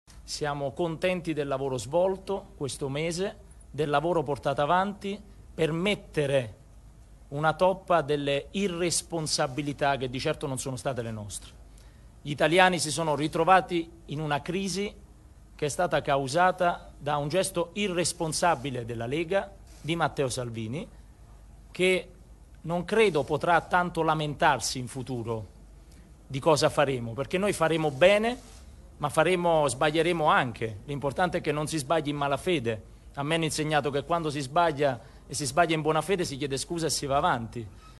Governo, Di Maio: "Abbiamo messo toppa alle irresponsabilità di Salvini"
Di Maio in conferenza stampa dopo il voto su Rousseau

POLITICA (Roma). "Gli italiani si sono ritrovati in una crisi che è stata causata da un gesto irresponsabile della Lega". Lo ha detto Luigi Di Maio alla Camera. (e.moretti)